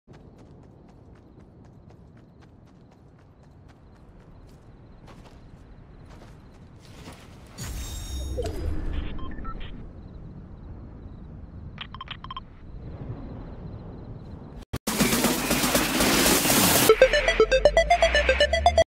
Where to search for a hidden gnome between Fancy view, a wooden shack, and a big house | Mission
Special thanks to PostHistoricGamer for the outro song for these videos! Link down below to their channel! Go show them some love!

My Xbox gamertag: CheapRabbi7321